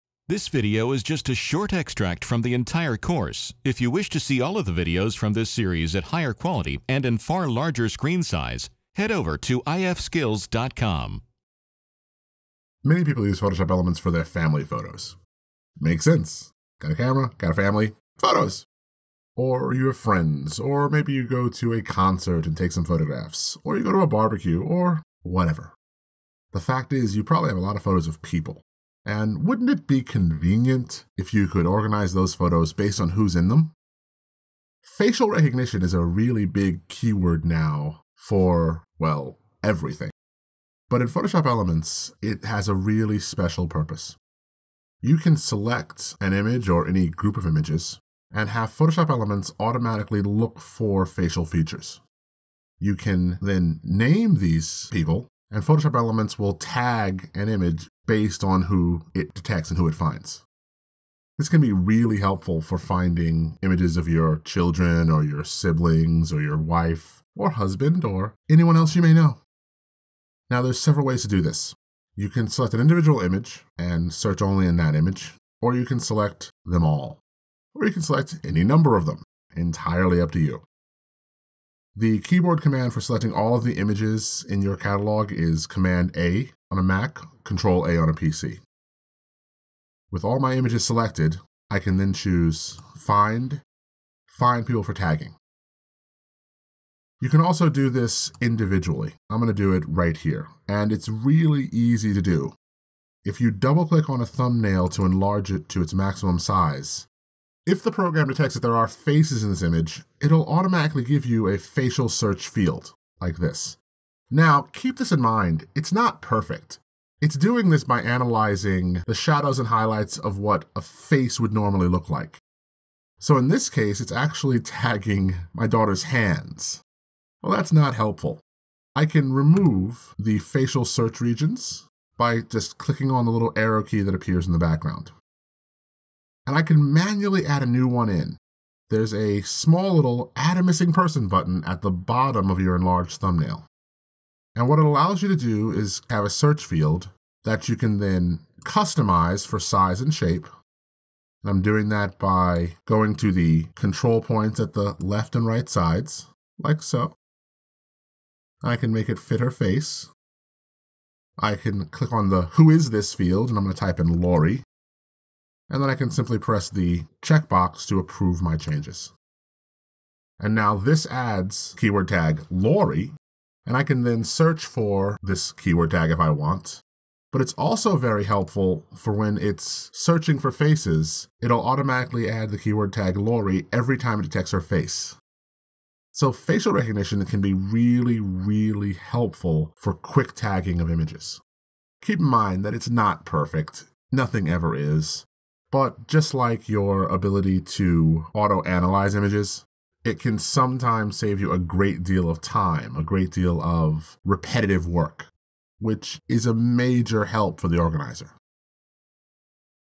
Adobe Photoshop Elements 9 - Finding People
This short Photoshop Elements 9 training video shows how to use the program's tagging and identification features to find people in your images. It's just a short, low resolution preview of the full tutorial on our website. YouTube